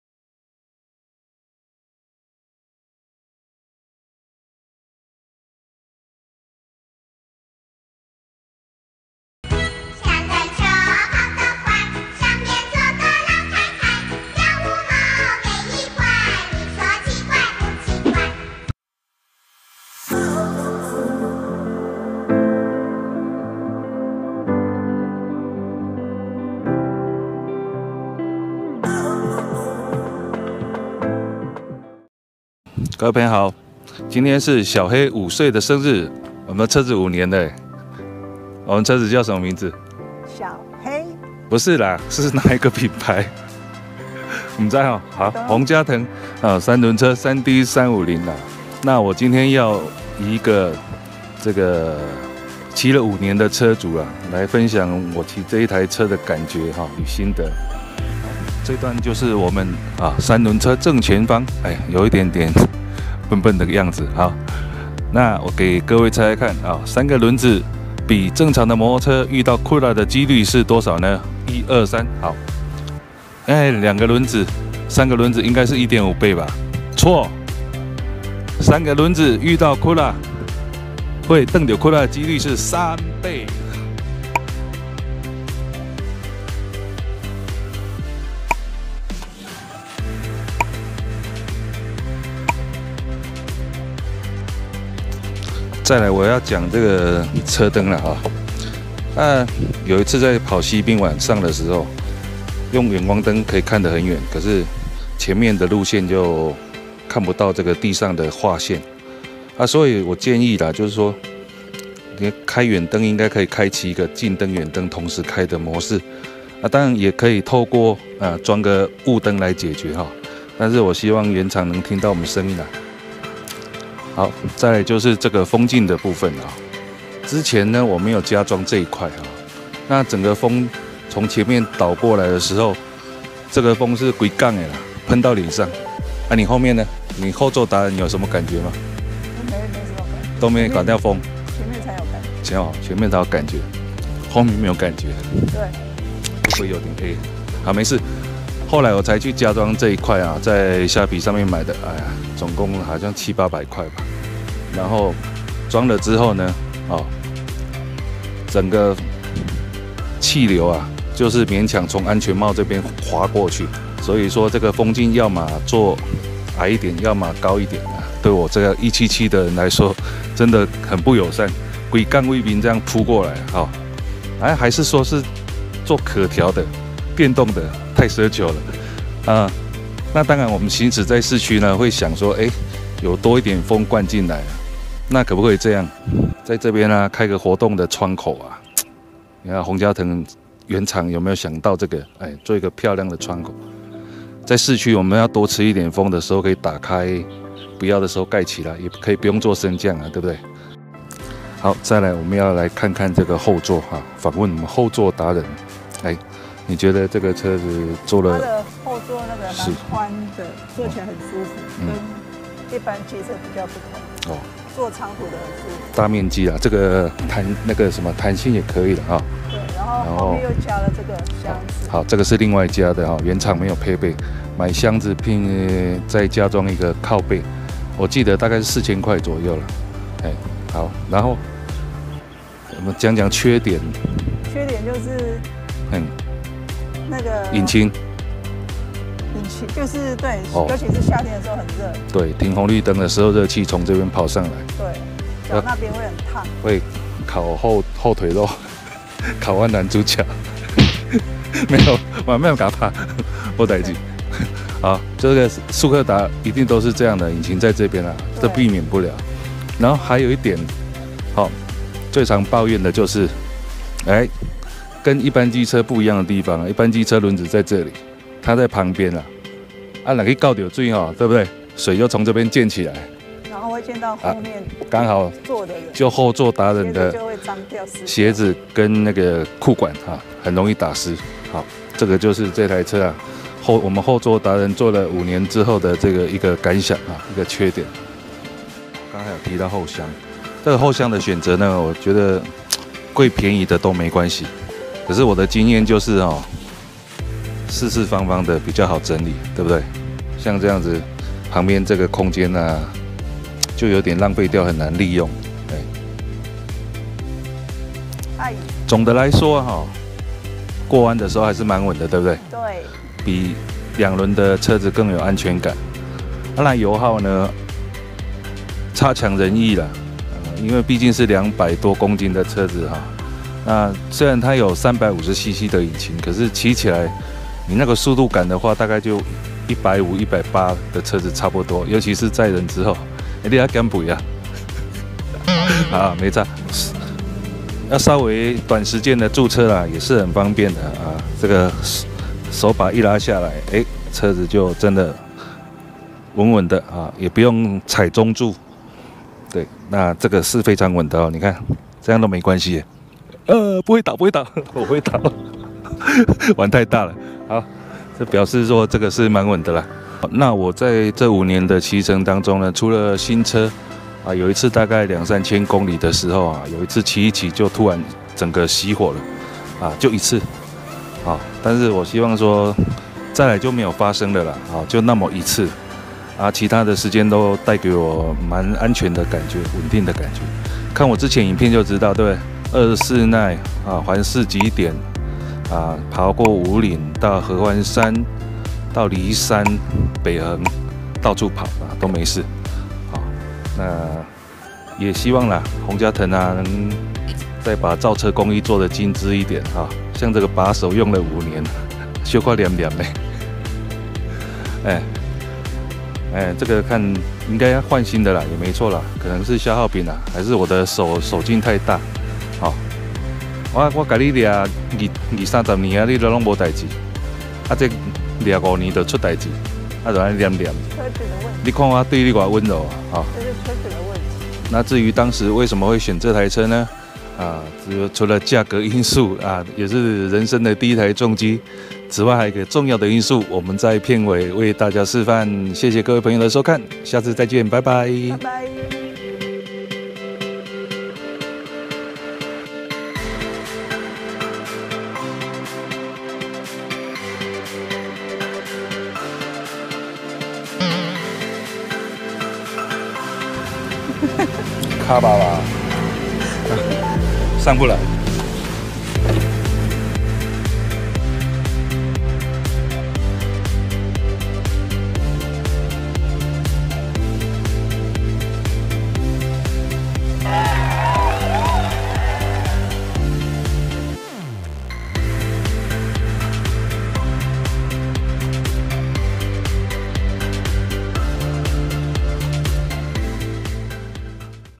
宏佳騰三輪機車 3D350 車主5年的騎乘感受分享
5年前. 我的車我的妻都好萌啊